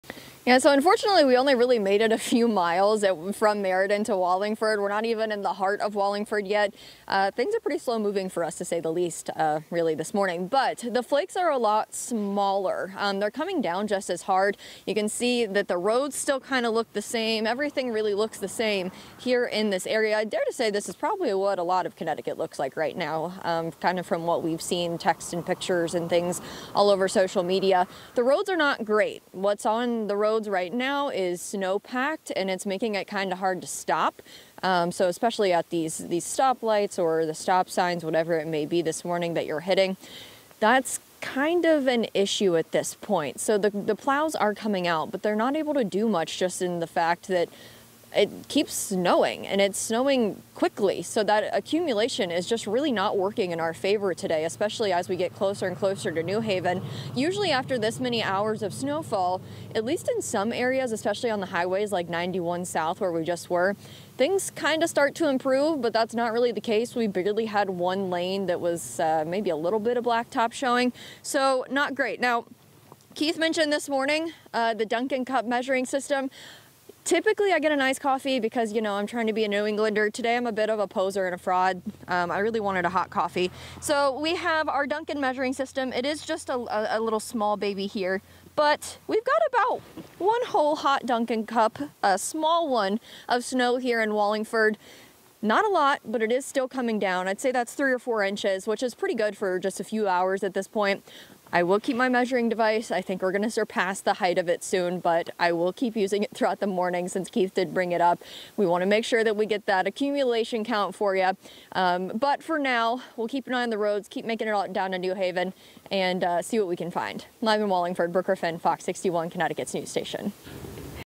How much snow is falling in Connecticut? Reporter measures with Dunkin' cup
Snow is piling up in Wallingford as the storm continues in Connecticut.